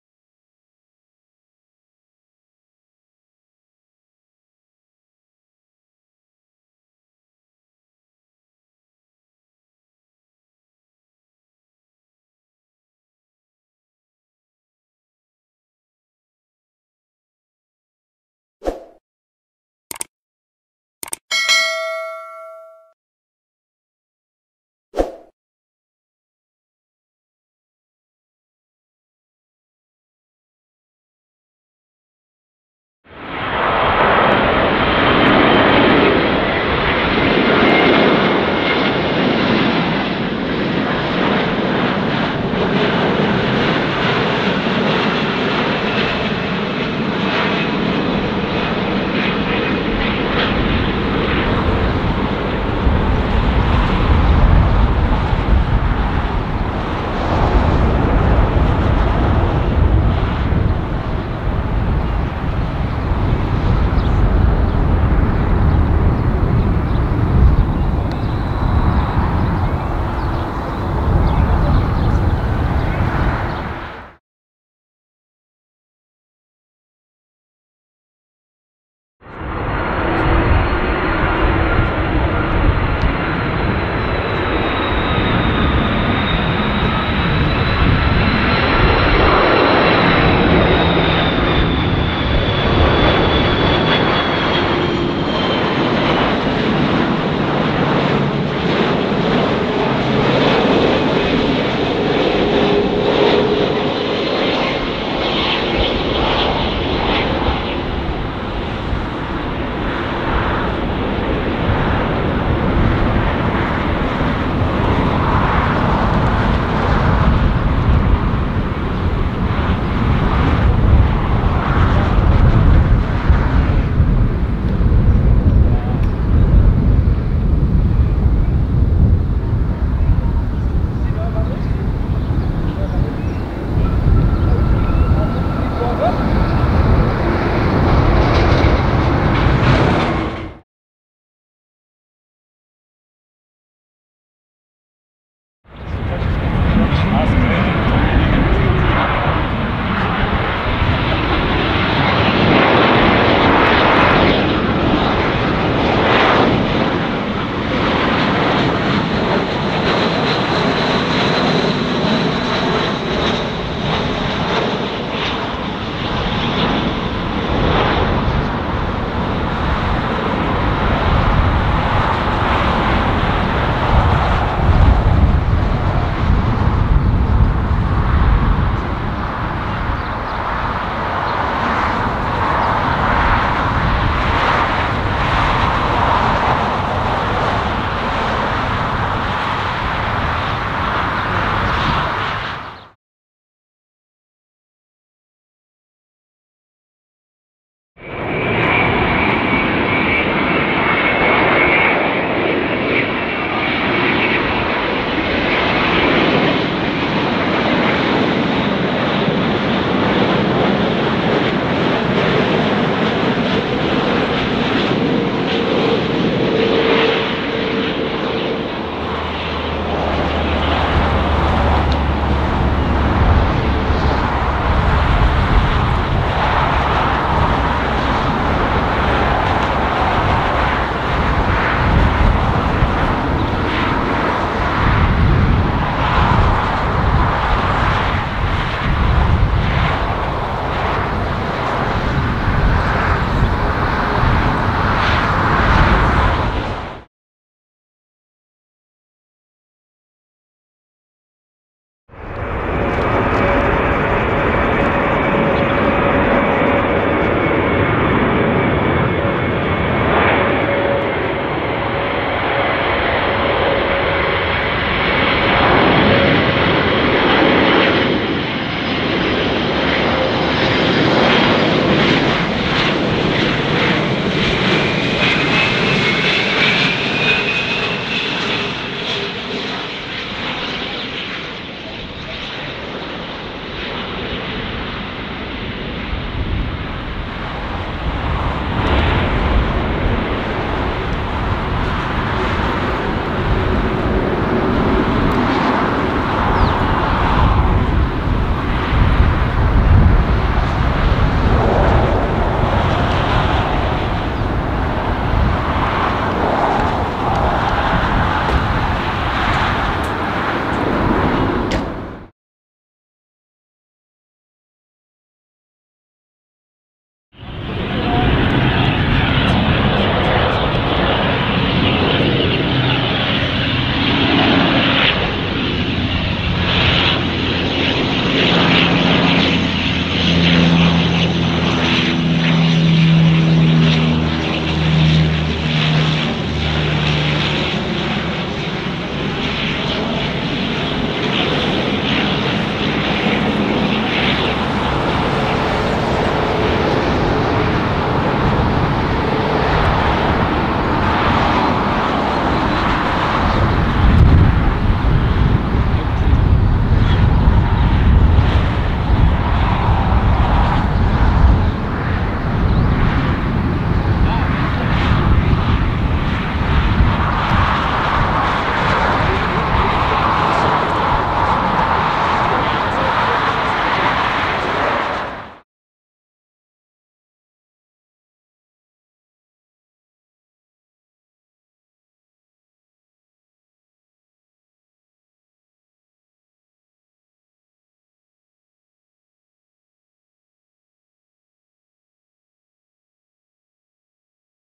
LANDINGS AT THE CHURCH - Afternoon Melbourne Airport Plane Spotting
Today Schony747 features arrivals at beside the church on Uniting Lane which is very good for afternoon arrivals as there's usually lots of action and the sun is in the right place.

00:00 - Introduction
00:27 - Qatar Airways Airbus A350-1041 landing from Doha
01:14 - Qantas Airbus A330-202 landing from Cairns
02:20 - Jetstar Airbus A320-232 landing from Adelaide
03:12 - Scoot Airways Boeing Dreamliner 787-9 landing from Singapore
04:07 - Volga Dnepr Antinov AN124-100 landing from Thailand
05:12 - Qantaslink De Havilland Dash 8 - 300 landing from Mildura

New Uploads Weekly - Trains, Planes, Trams

Filmed 29th December 2021 Operations Road Melbourne Airport
Filmed in 4K Sony AX53